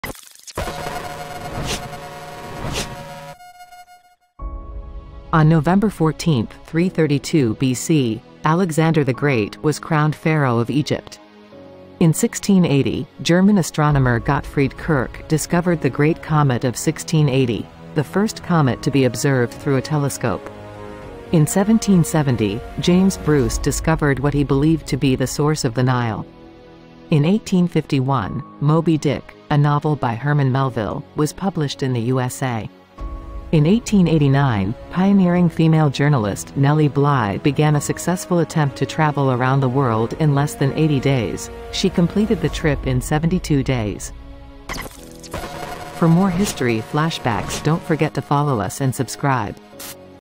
Flashback In History: November 14
On November 14, 332 BC, Alexander the Great was crowned pharaoh of Egypt.

In 1680, German astronomer Gottfried Kirch discovered the Great Comet of 1680, the first comet to be observed through a telescope.

In 1770, James Bruce discovered what he believed to be the source of the Nile.

In 1851, "Moby-Dick," a novel by Herman Melville, was published in the USA.

In 1889, pioneering female journalist Nellie Bly (aka Elizabeth Cochrane) began a successful attempt to travel around the world in less than 80 days. She completed the trip in 72 days.